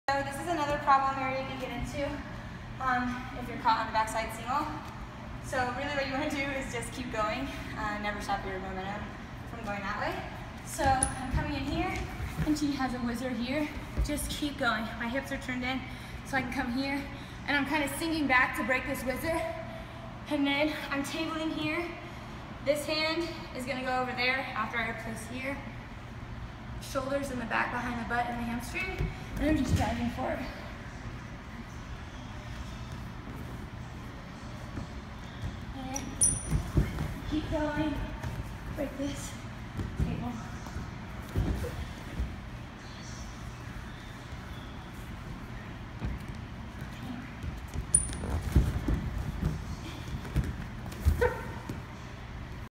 Finishing single leg on the backside
Sarah Hildebrandt goes over how to finish your single from the backside when stuck on the mat with Erin Golston